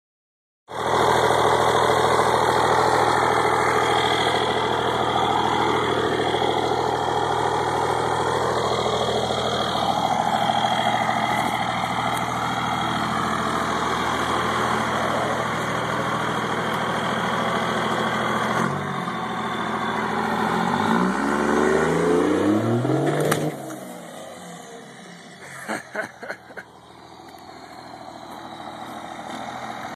Stump Pulling
This is how the brothers Grove pull stumps.  I skipped the boring part of cutting off most of the branches and saved the best for the camera.
Specs on the truck:  1999 Ram 2500, diesel, 6-speed, 4" straight pipe, Quadzilla Adrenaline (PL2 for this demonstration).  First attempt, 3rd gear, low-range.